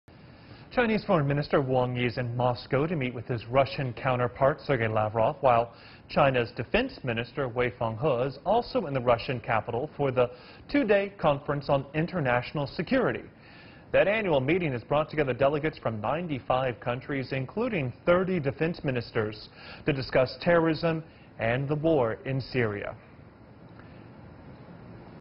China's foreign, defense ministers in Moscow
Chinese Foreign Minister Wang Yi is in Moscow to meet with his Russian counterpart, Sergey Lavrov. While China's defense minister, Wei Fenghe, is also in the Russian capital for the two-day Conference on International Security.